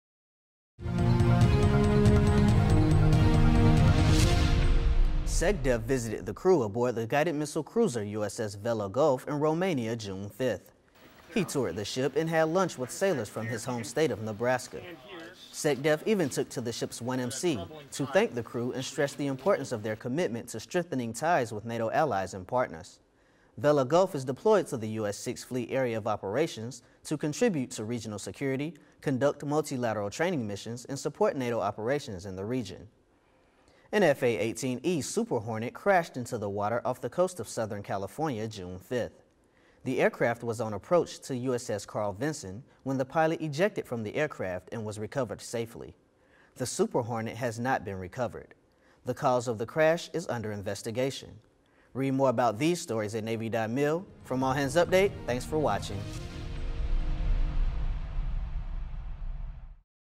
SECDEF Visits USS Vella Gulf in Romania; F/A-18 Crashes, Pilot Stable
All Hands Update Friday, June 6, 2014 #1